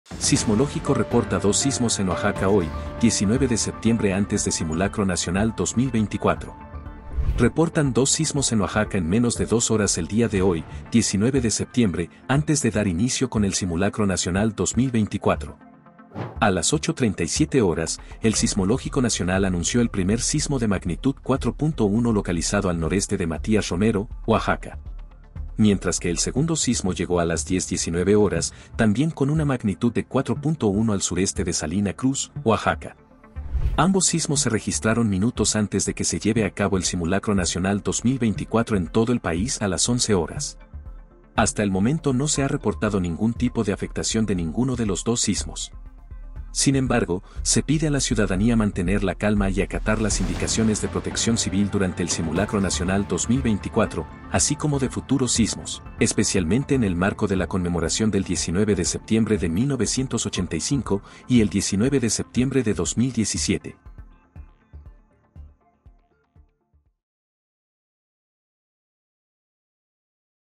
Sismológico reporta DOS SISMOS en Oaxaca HOY, 19 de septiembre antes de Simulacro Nacional 2024
Reportan dos sismos en Oaxaca en menos de dos horas el día de hoy, 19 de septiembre, antes de dar inicio con el Simulacro Nacional 2024.

A las 8:37 horas, el Sismológico Nacional anunció el primer sismo de magnitud 4.1 localizado al noreste de Matías Romero, Oaxaca.

Mientras que el segundo sismo llegó a las 10:19 horas, también con una magnitud de 4.1 al sureste de Salina Cruz, Oaxaca.

Ambos sismos se registraron minutos antes de que se lleve a cabo el Simulacro Nacional 2024 en todo el país a las 11:00 horas.